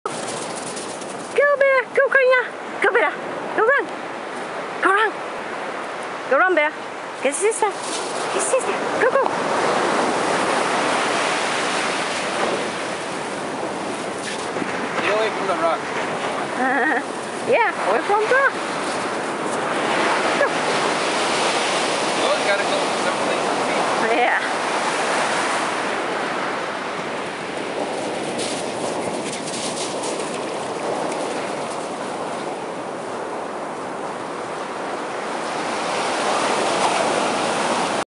IG Bella and Carina's day trip to Santa Barbara Dog Beach
They love dog beaches, especially Bella is more active when we take her to the beach.